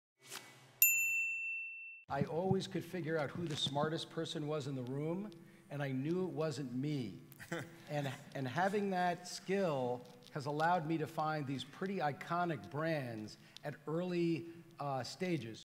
Greg Coleman, president at Buzzfeed in 13 seconds during Entrepreneur Weekend
The panel included:

Jessica Alba, founder of The Honest Company
Neil Blumenthal, co-founder and co-CEO of Warby Parker
Greg Coleman, president of Buzzfeed
Serial entrepreneur and entertainer MC Hammer
Jennifer Hyman, co-founder and CEO of Rent the Runway
Dan Rosensweig P’15’17, CEO of Chegg!